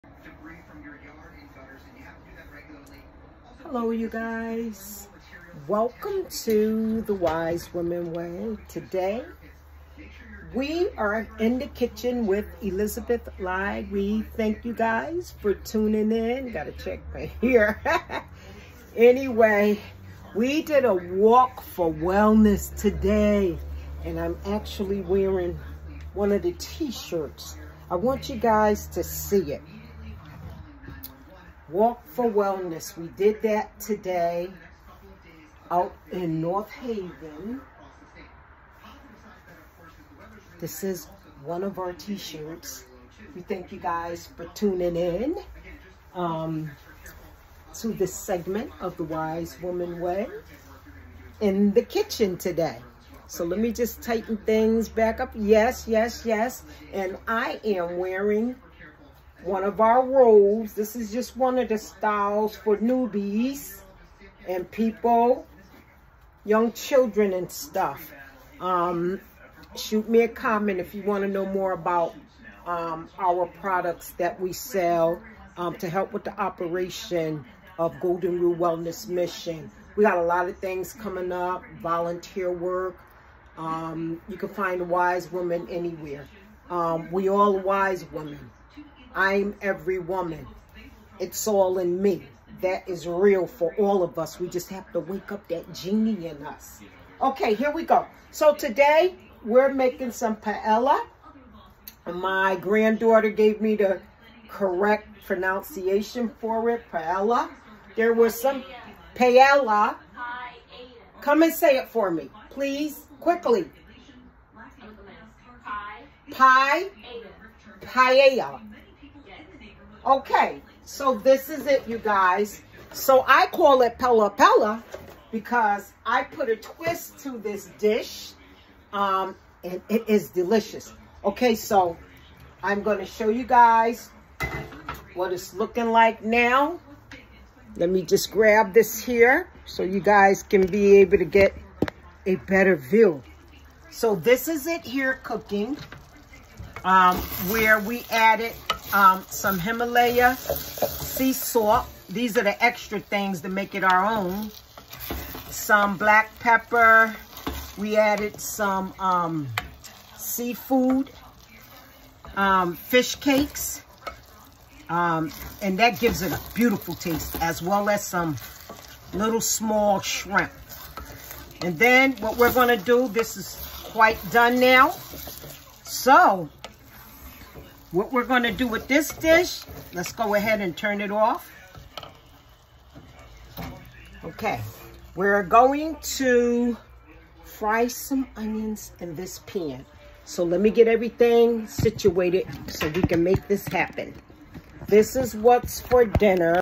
From Goya Paella to Tresa’s Pella Pella Dish
We shared how we make Paella our own Pella Pella seafood dish. This time we added more seafood and other ingredients to make it our own.